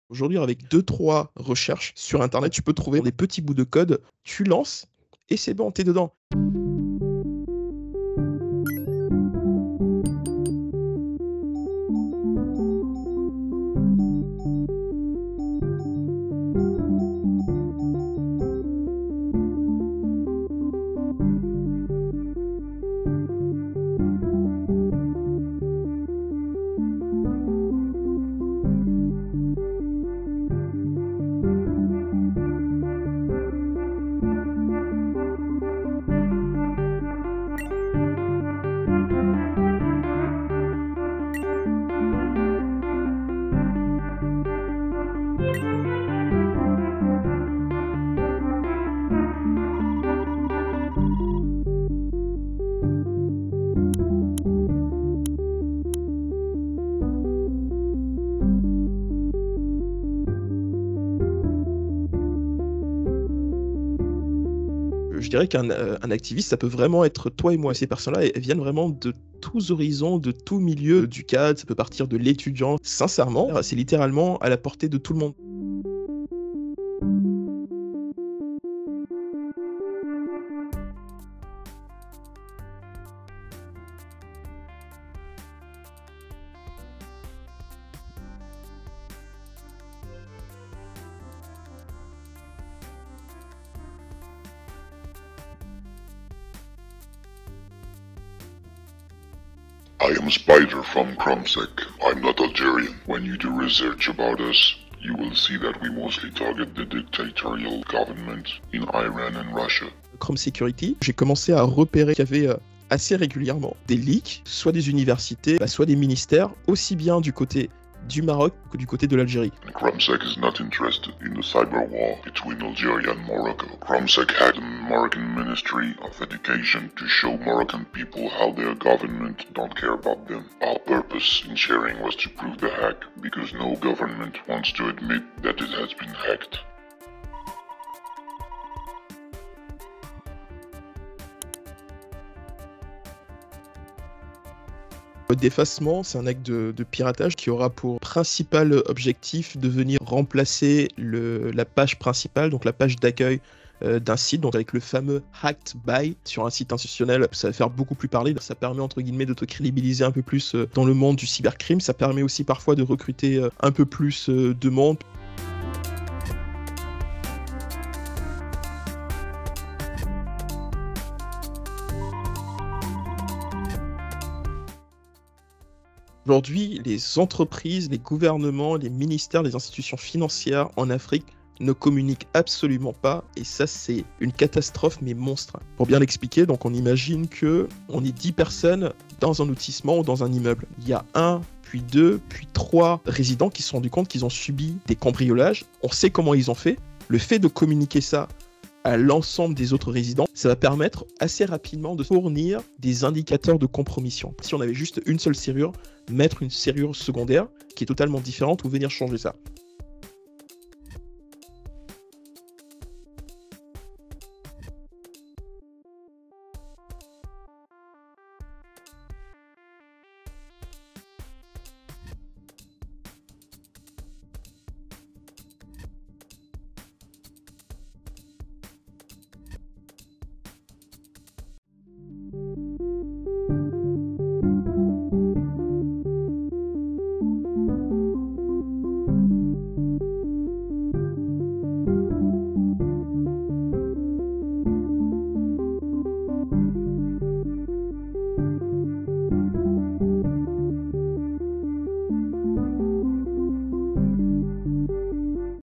Espionnage et « hacktivisme » : la cyberguerre entre le Maroc et l’Algérie
Depuis 2020, les institutions gouvernementales marocaines et algériennes sont régulièrement attaquées par des collectifs d’« hacktivistes ». Une guerre numérique à la lisière de l’espionnage et de la cybercriminalité.
Qui sont les groupes qui mènent ces attaques ? Comment opèrent-ils ? Quelle menace représentent-ils pour la sécurité numérique au Maghreb ? Décryptage en vidéo.